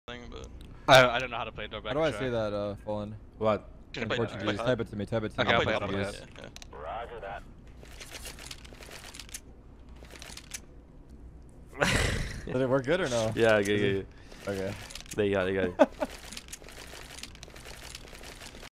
tarik meteu essa antes de ganhar de 16x2 da mibr
clipe retirado da stream: /gafallen